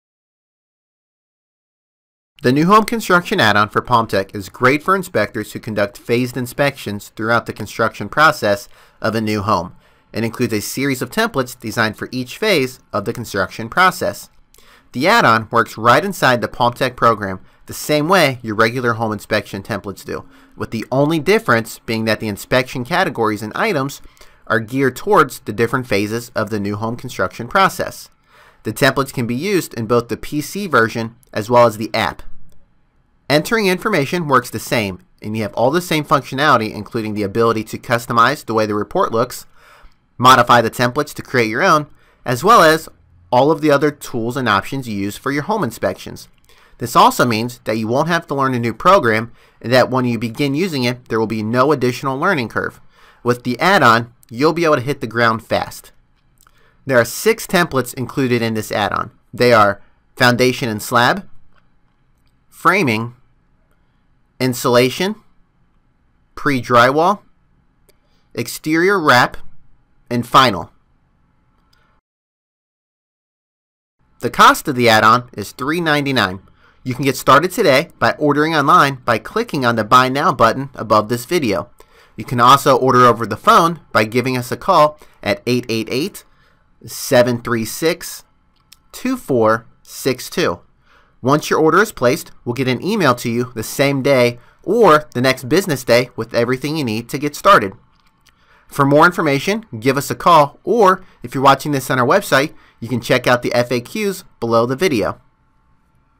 Palm-Tech 7: New Home Construction Add-on
This video goes over the New Home Construction add-on available with Palm-Tech Home Inspection Software.